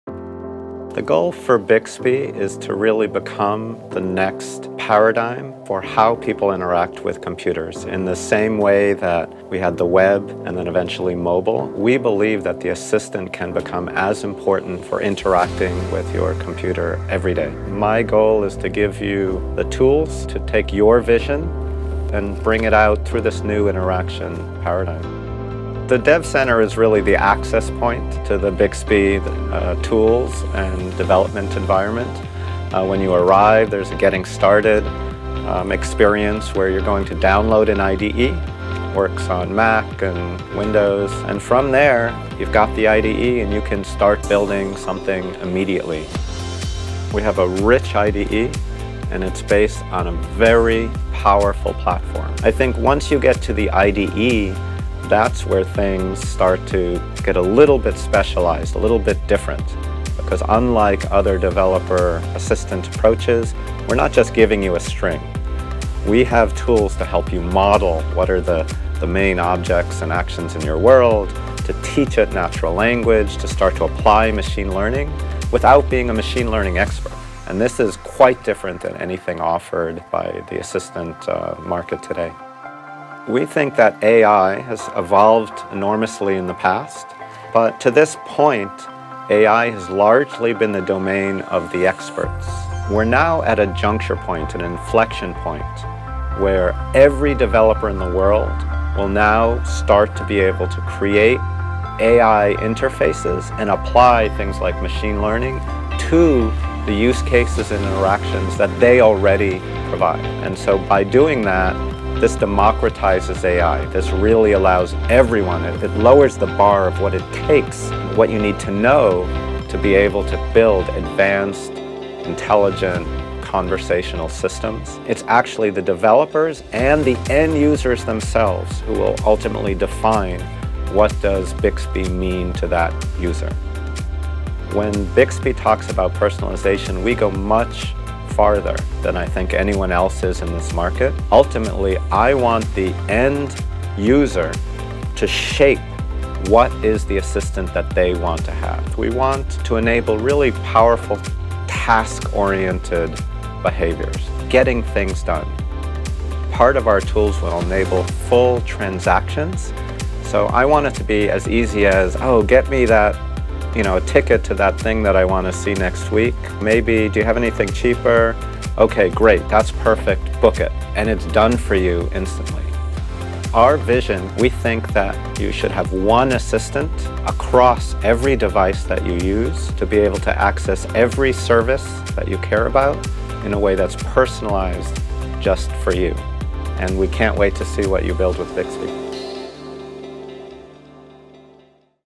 Bixby Tools
A new way of building software

All of the Bixby tools and workflows you need are in one place making it easier to build, test, and publish to the Bixby Marketplace. The best part: you can integrate your existing service without needing to be an expert in AI.